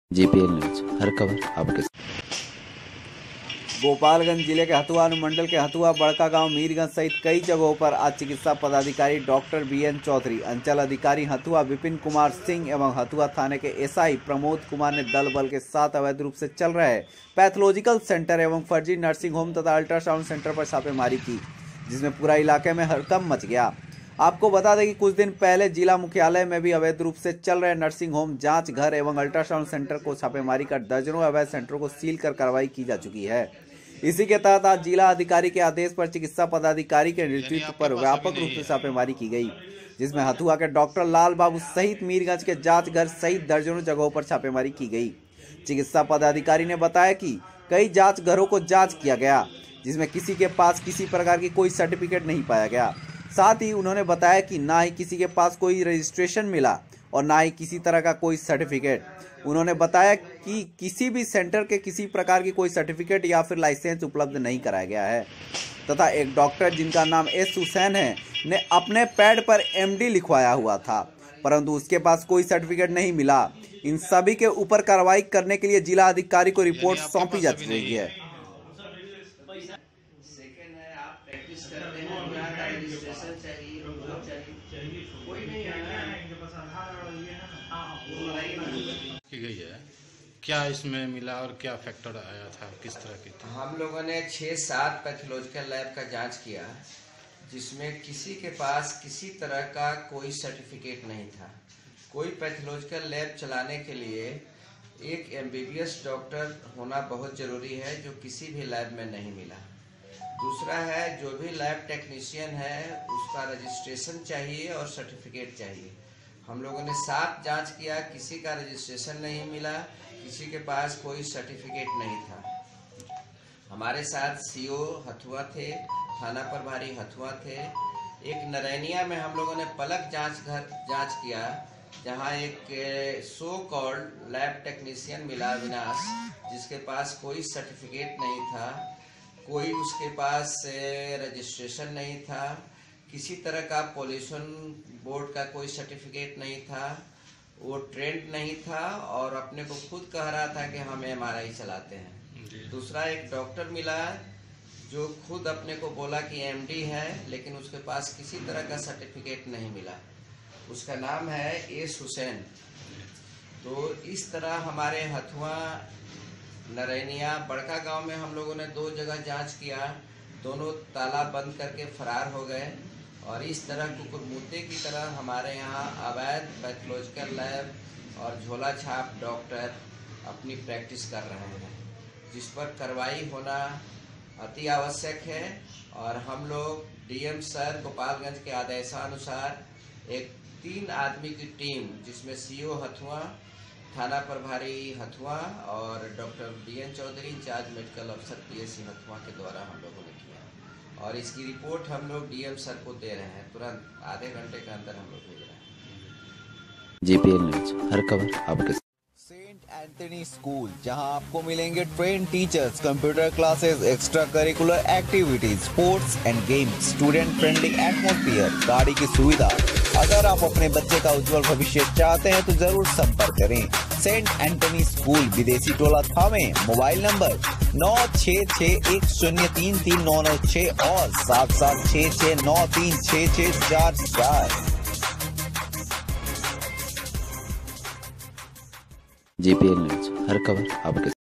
बिना सर्टिफिकेट और रजिस्ट्रेशन से चल रही है पैथोलॉजिकल सेंटर, नर्सिंग होम तथा अल्ट्रासाउंड सेंटर,
जिले के हथुआ अनुमंडल के हथुआ, बड़कागांव ,मीरगंज सहित कई जगहों पर आज चिकित्सा पदाधिकारी डॉ बी एन चौधरी अंचल अधिकारी हथुआ विपिन कुमार सिंह एवं हथुवा थाने के एस आई प्रमोद कुमार ने दल बल के साथ अवैध रूप से चल रहे पैथोलॉजिकल सेंटर एवं फर्जी नर्सिंग होम तथा अल्ट्रासाउंड सेंटर पर छापेमारी की जिससे पूरा इलाके में हड़कंप मच गया । आपको बता दें कि कुछ दिन पहले जिला मुख्यालय में भी अवैध रूप से चल रहे नर्सिंग होम जांच घर एवं अल्ट्रासाउंड सेंटर को छापेमारी कर दर्जनों अवैध सेंटरों को सील कर कार्रवाई की जा चुकी है । इसी के तहत आज जिला अधिकारी के आदेश पर चिकित्सा पदाधिकारी के नेतृत्व में व्यापक रूप से छापेमारी की गई जिसमें हथुआ के डॉ लाल बाबू सहित मीरगंज  में जांच घर सहित दर्जनों जगहों पर छापेमारी की गई  । चिकित्सा पदाधिकारी ने बताया कि कई  जांच घरो को  जांच किया गया जिसमें किसी के पास किसी प्रकार का कोई सर्टिफिकेट नहीं पाया गया।  साथ ही उन्होंने बताया कि ना ही किसी के पास कोई रजिस्ट्रेशन मिला और ना ही किसी तरह का कोई सर्टिफिकेट । उन्होंने बताया किकिसी भी सेंटरो पर किसी प्रकार का कोई सर्टिफिकेट या फिर लाइसेंस उपलब्ध नहीं कराया गया । तथा एक डॉक्टर जिसका नाम  एस हुसैन है ने अपने पैड पर  एम डी लिखवाया हुआ था परंतु उसके पास कोई सर्टिफिकेट नहीं मिला इन सभी के ऊपर कारवाई करने के लिए जिला अधिकारी को रिपोर्ट सौंपी जा रही है।